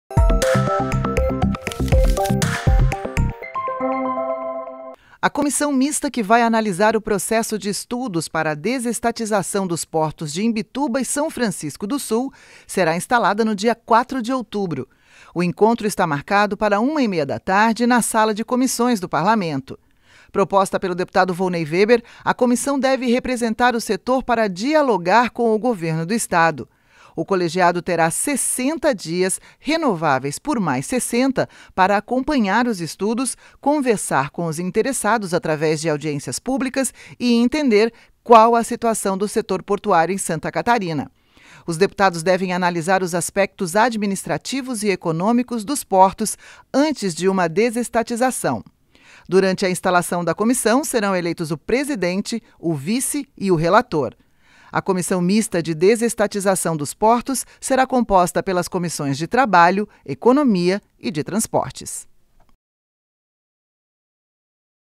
É Notícia - Comissão mista vai acompanhar desestatização dos portos - 01/10/21
Será instalada na próxima segunda-feira (4), uma comissão mista que vai apreciar e analisar o processo de estudos para a desestatização dos portos de Imbituba e de São Francisco do Sul. O colegiado será composto pelas comissões de trabalho, economia e de transportes.